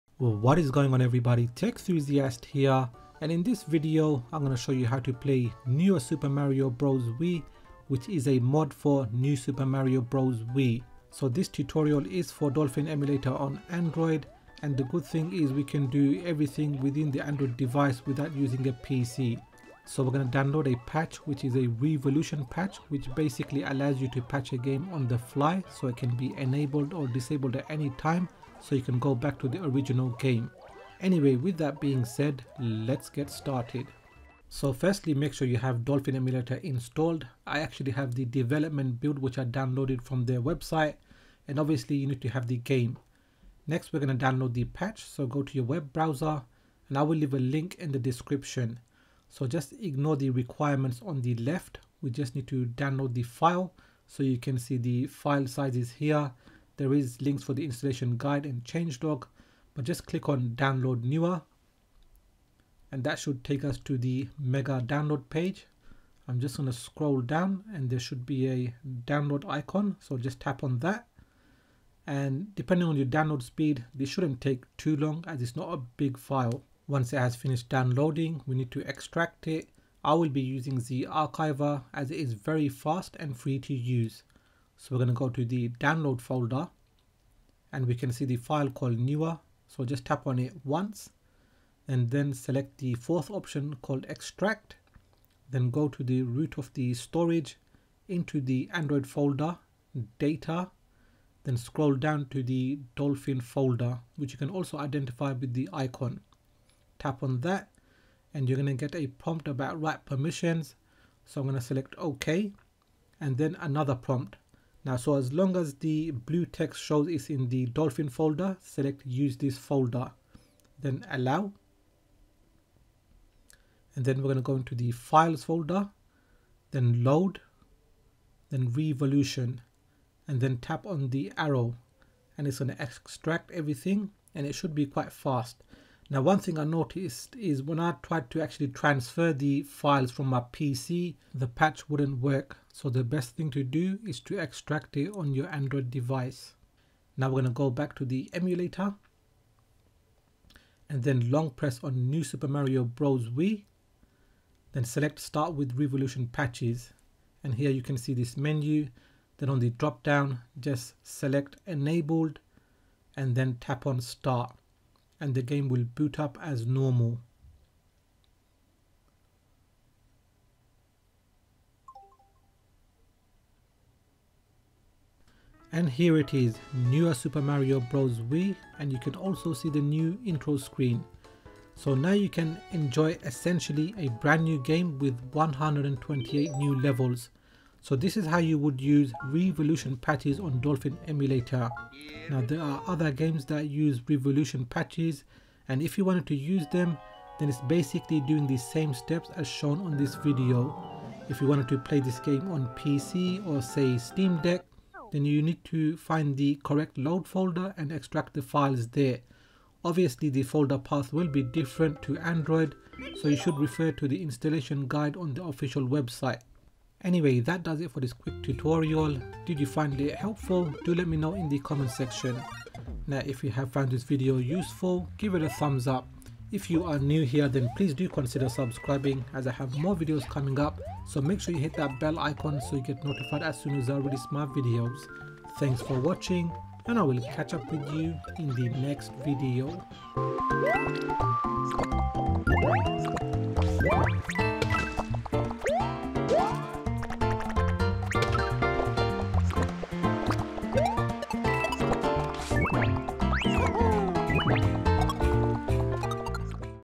How to add Riivolution patches on Dolphin Emulator (Android)
Riivolution is an on-the-fly game patcher that allows you to use custom textures, music, levels, memory patches, game translations, and more!